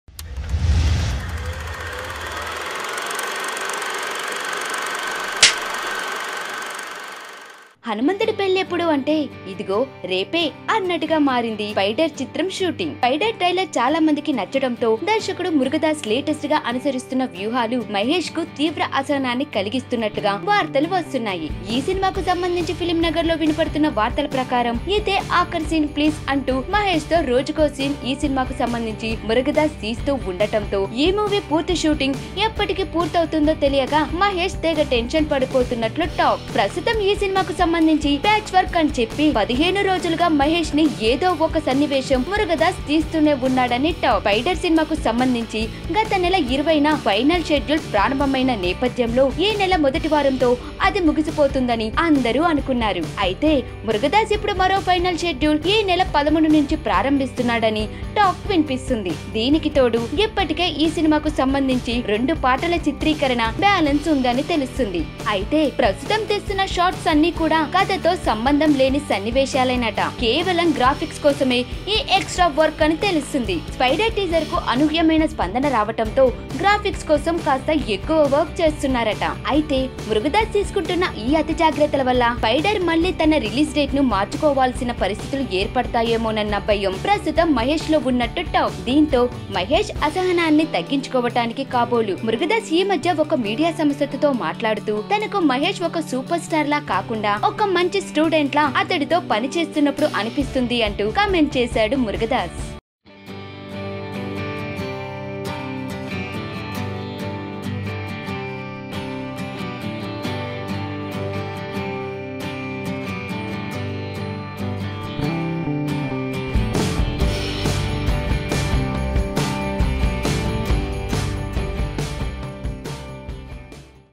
Director Irritates Mahesh Babu On Spyder Movie Shooting | Latest Filmy News
Watch Director Irritates Mahesh Babu On Spyder Movie Shooting | Latest Filmy News